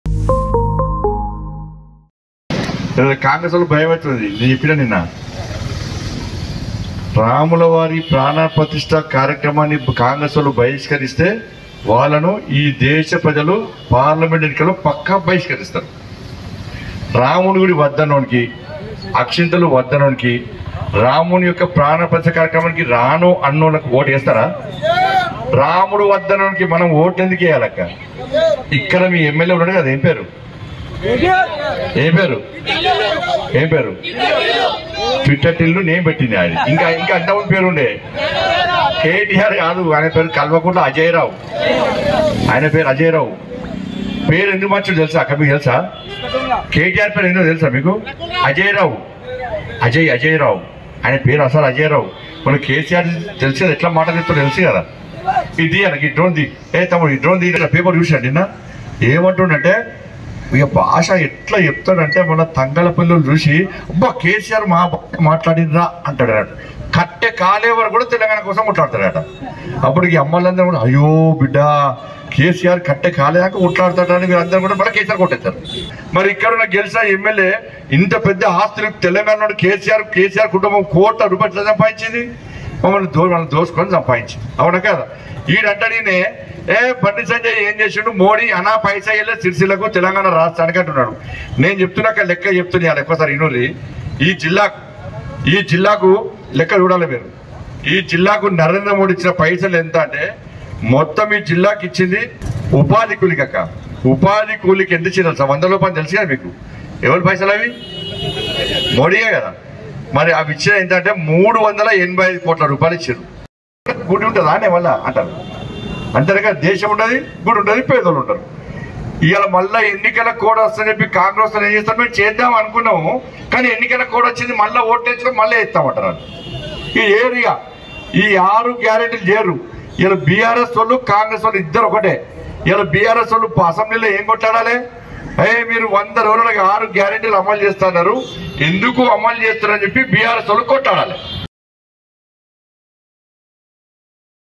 BJP MP Bandi Sanjay Sensational Comments On KTR : ఆయన పేరు కేటీఆర్ కాదు.... ఆయన అసలు పేరు..?
BIG TV Karimnagar Is Part Of The BigTV Telugu It Is A Technology First 24/7 LIVE Telugu Satellite News Channel.